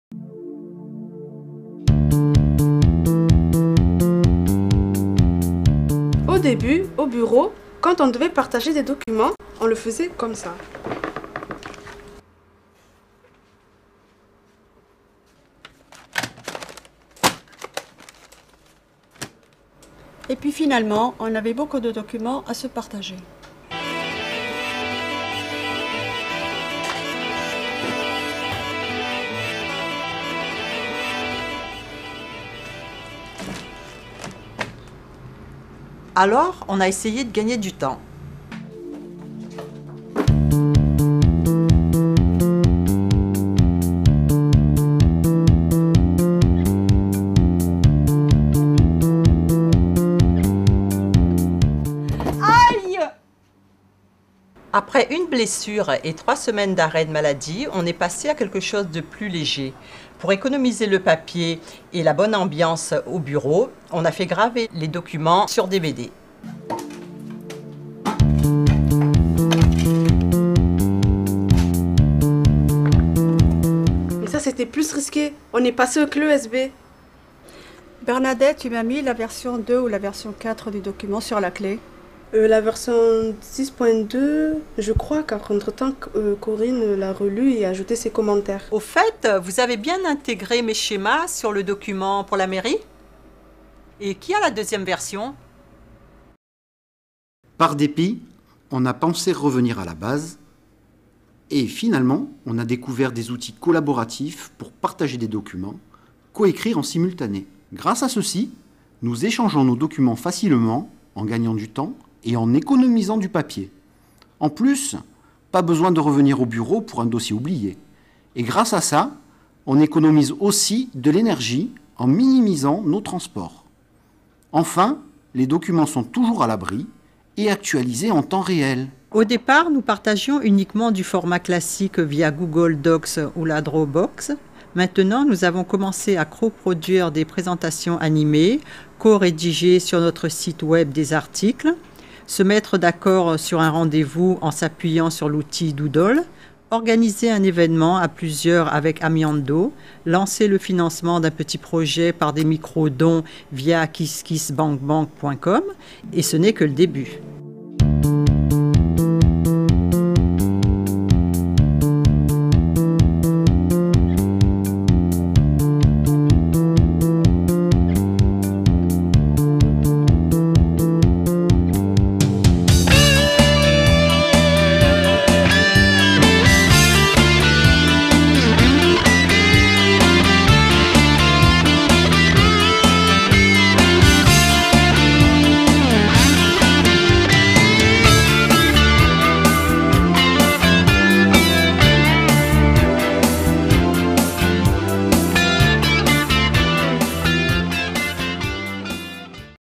Soyons Eco Responsable : Clip Gestion Humaine et Outils Collaboratifs
Les gestes éco responsables sont importants pour notre avenir. Voici un clip réalisé pour découvrir des outils collaboratifs pour limiter l'utilisation du papier par exemple.